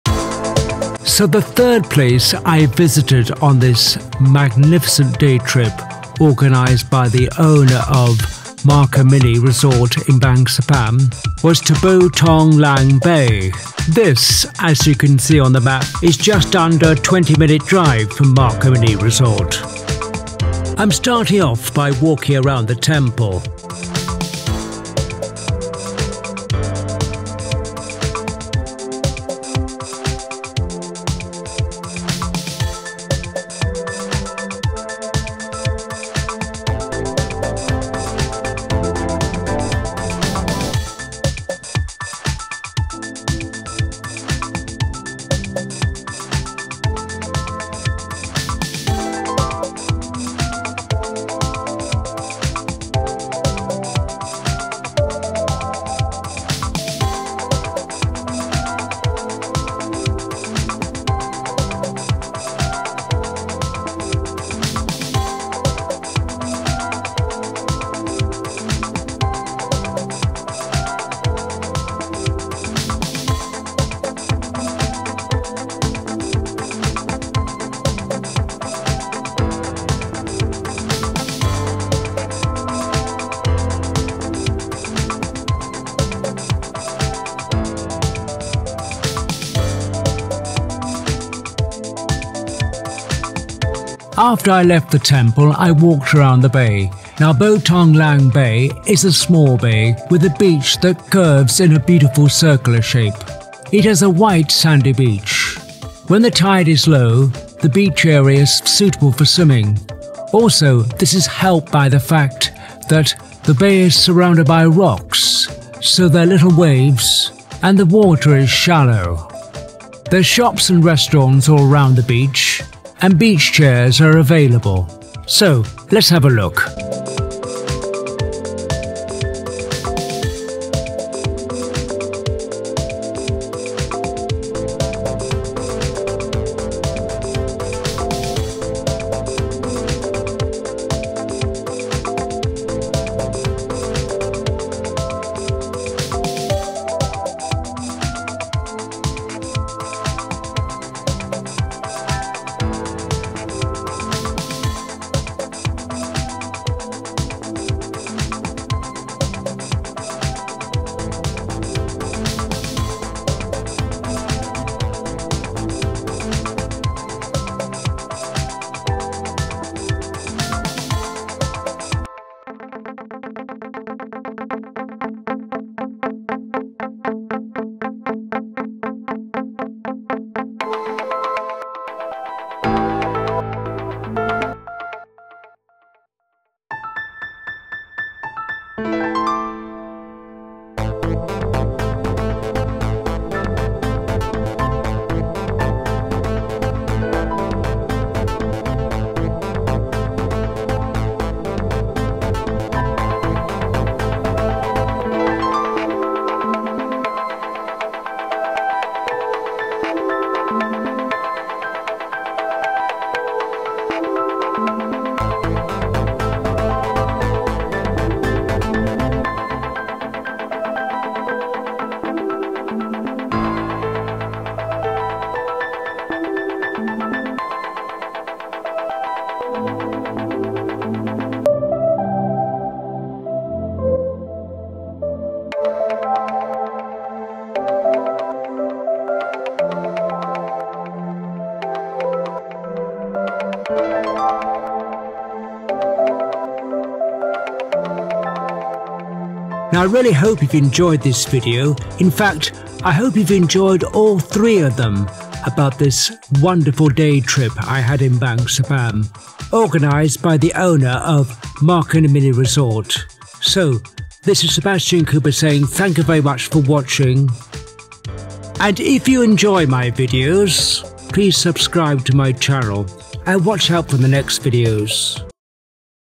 Bo Tong Lang Bay, Bang Saphan Province, Thailand
Bo Tong Lang Bay is a small bay with a beach that curves in a beautiful circular shape. It has a white sandy beach. When the tide is low, the beach area is suitable for swimming. The water is shallow and the bay is surrounded by rocks, so there are only little waves. There is a rocky island in the middle of the bay. Shops and restaurants are around the beach and beach chairs is available for tourists.